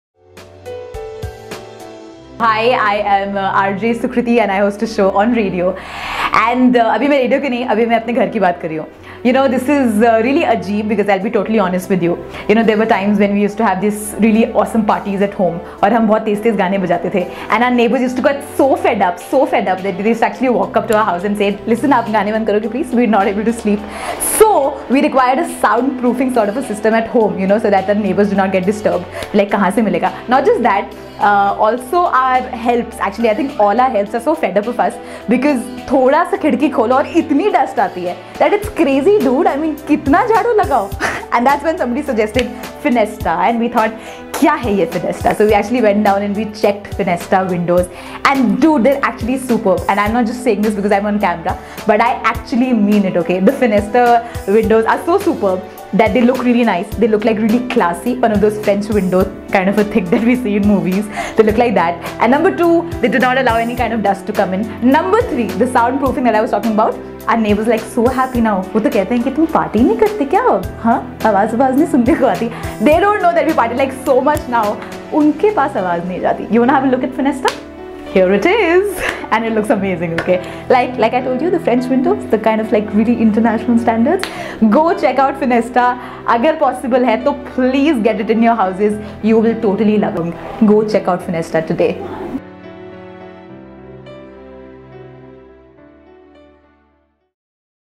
RJ Sukriti, Jalvayu Vihar , Sec 21, Noida
"Our neighbors were so fed-up of our loud late night parties and our house help was tired of constantly cleaning the house, to keep the dust away. Then I got to know about Fenesta Windows, which are totally superb, they look really nice and classy, they don’t allow any dust to come inside and our house is sound proof now. Go check out Fenesta Windows today. “ – RJ Sukriti, Jalvayu Vihar , Sec 21, Noida